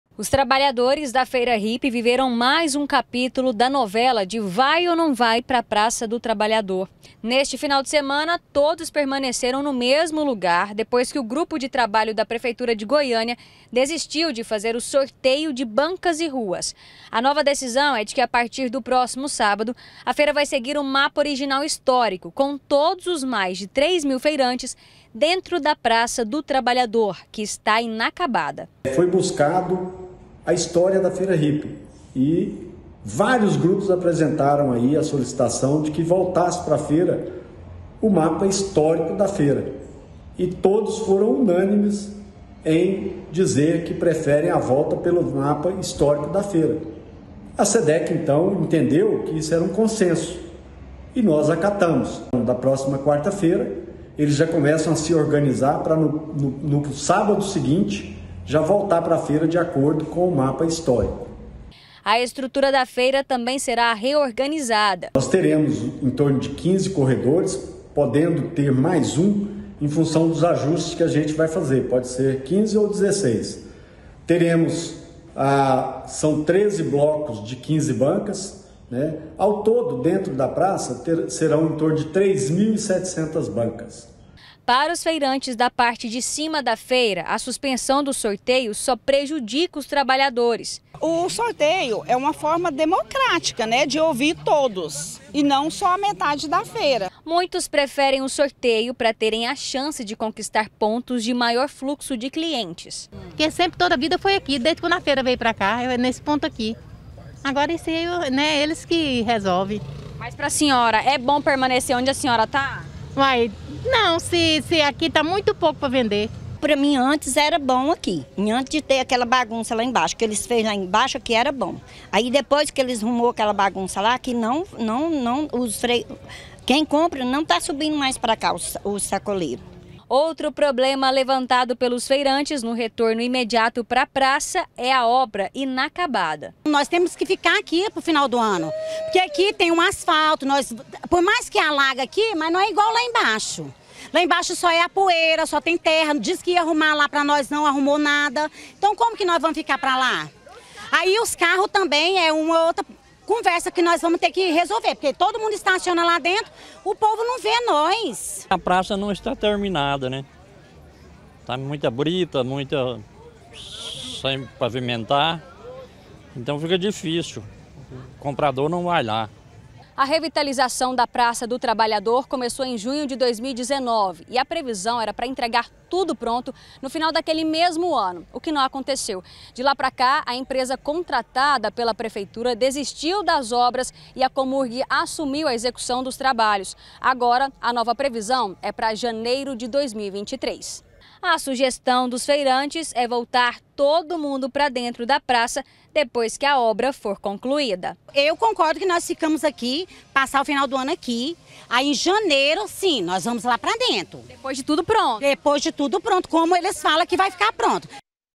FEIRA HIPPIE NÃO DEVE IR PARA PRAÇA DO TRABALHADOR
AQUELA ANTIGA POLÊMICA DA FEIRA HIPPIE TÁ DE VOLTA. A NOVIDADE É QUE A PREFEITURA DECIDIU QUE A FEIRA AINDA NÃO VAI MUDAR PRA PRAÇA DO TRABALHADOR E VAI FUNCIONAR NO MESMO LOCAL QUE ESTÁ HÁ TRÊS ANOS. A DECISÃO FOI TOMADA DEPOIS DE AVALIAÇÕES DE QUE NÃO HAVIA TEMPO PARA FAZER REFORMAS EM UM NOVO LUGAR.